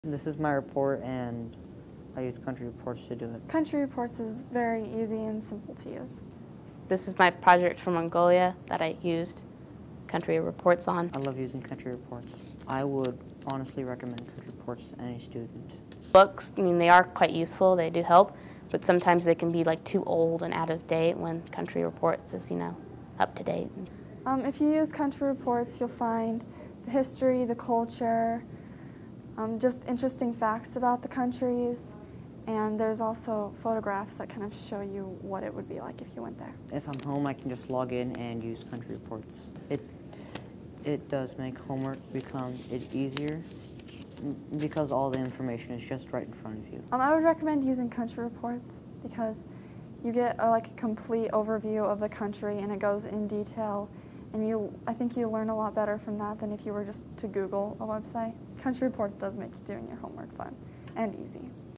CountryReports: Testimonial
Students explain why they prefer using CountryReports.